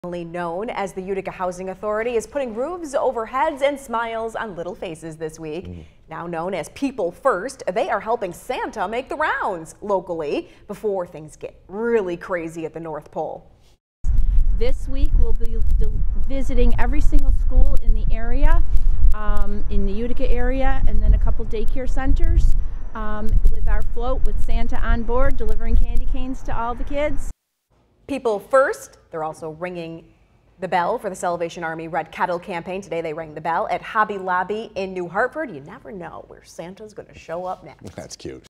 'People First' Helping Santa Make the Rounds
A local organization is delivering smiles and holiday cheer to area children this joyful season.